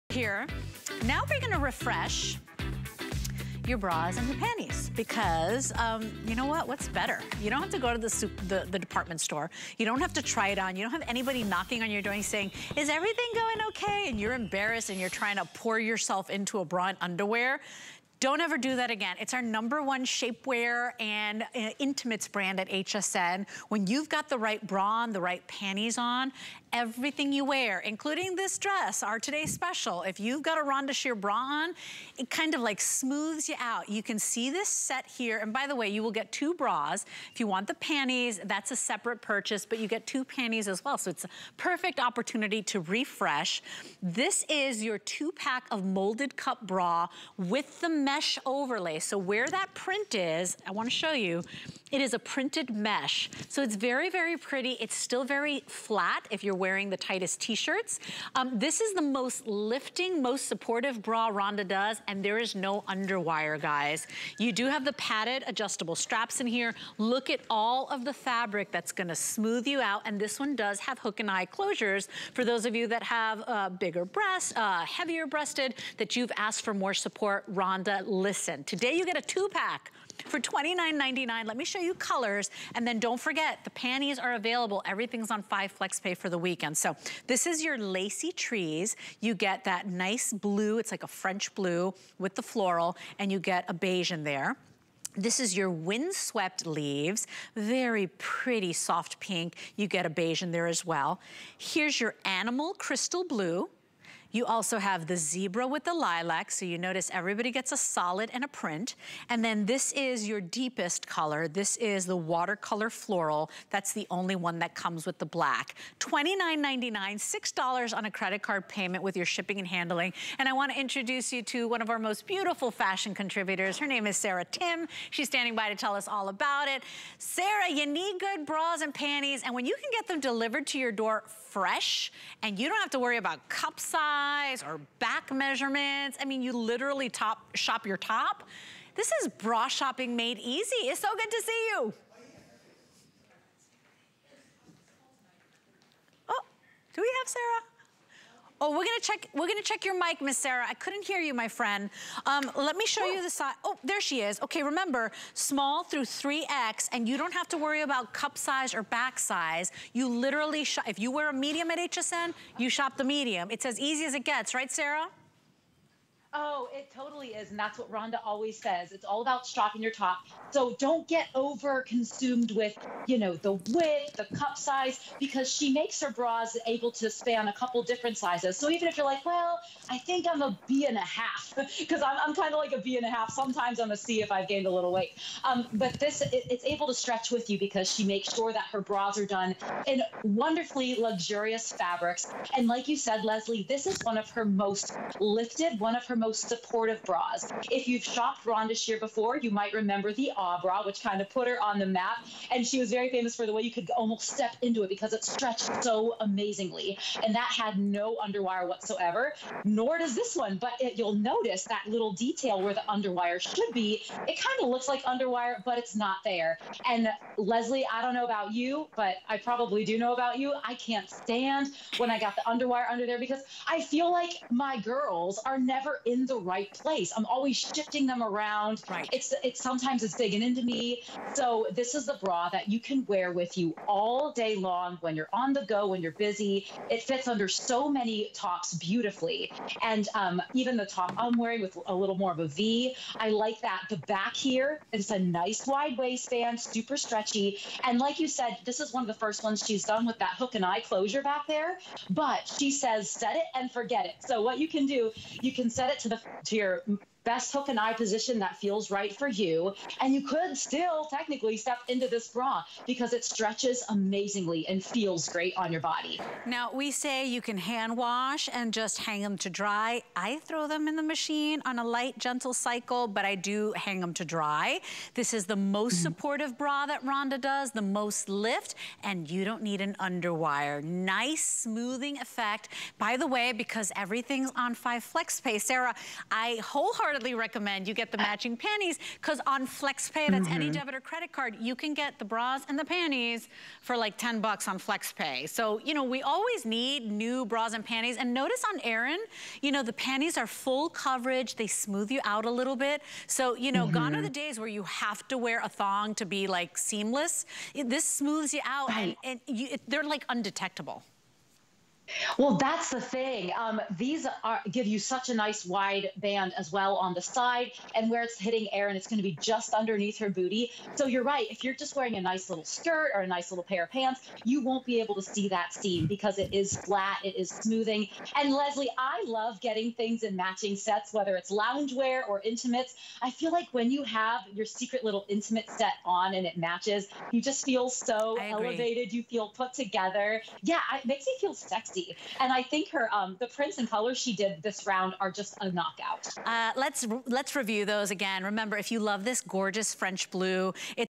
Rhonda Shear 2pack Molded Cup Bra with Mesh Overlay
Define your curves with Rhonda Shear's shapely, yet comfortable, moldedcup, wirefree bra. Adjust the padded straps for the ideal fit.

    1 printed bra
    1 solid bra